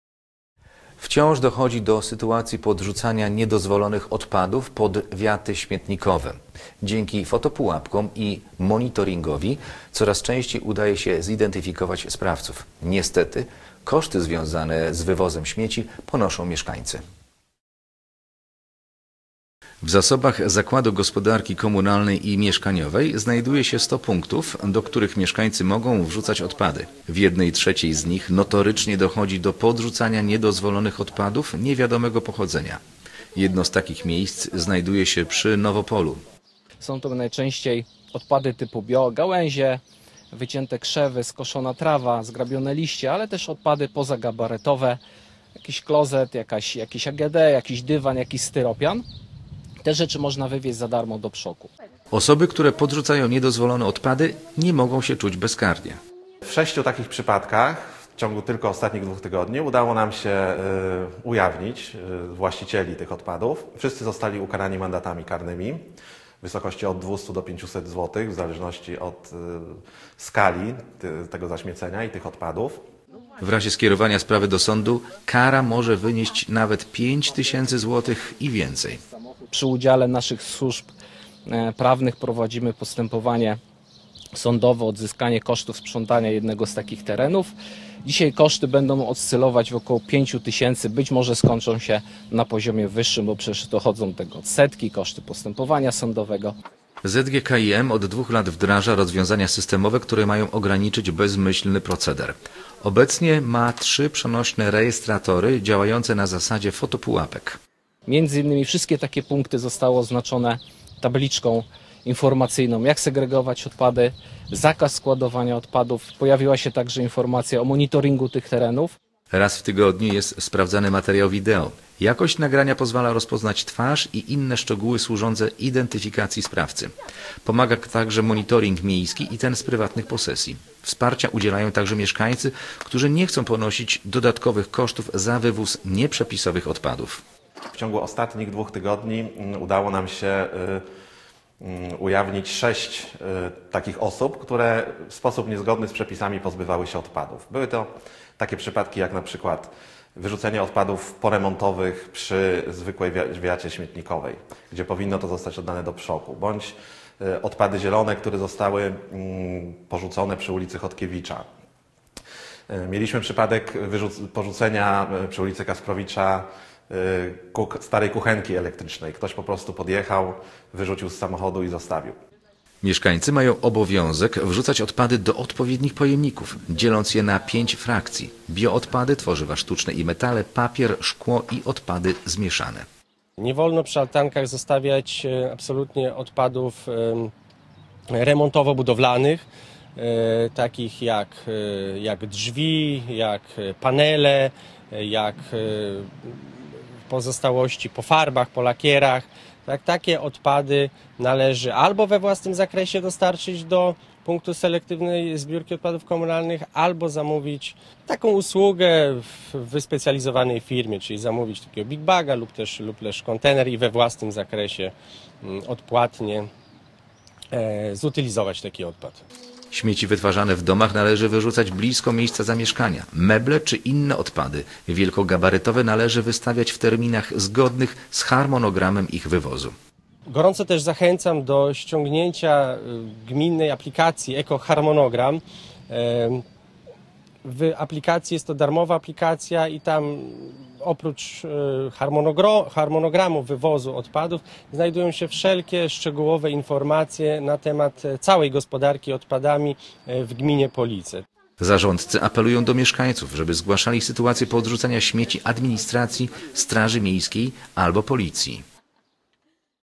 Nawet 5 tys. złotych kary mogą otrzymać osoby podrzucające niedozwolone odpady pod wiaty śmietnikowe
Teletydzień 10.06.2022 - Wciąż dochodzi do sytuacji podrzucania niedozwolonych odpadów pod wiaty śmietnikowe. Dzięki fotopułapkom i monitoringowi coraz częściej udaje się zidentyfikować sprawców. Niestety koszty związane z wywozem śmieci ponoszą mieszkańcy